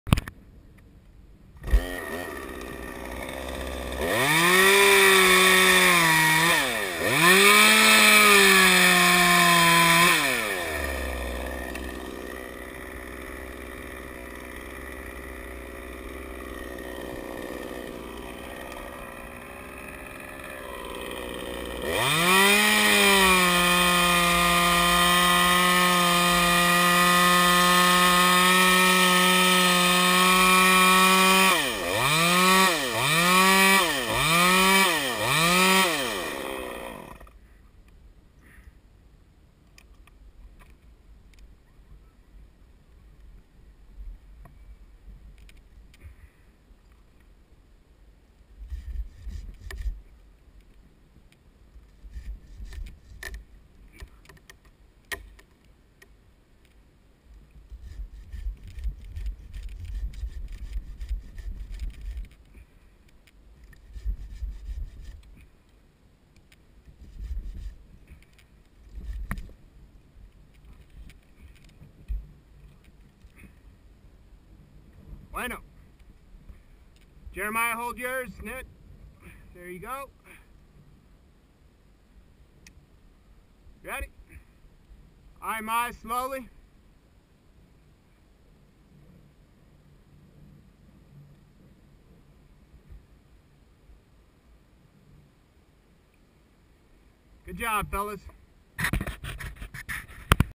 maple log in Bellfountaine.MP4
Rigging the log of a long Silver Maple limb over  4k.v. electrical lines using a DMM Pinto Rig Pulley at the anchor point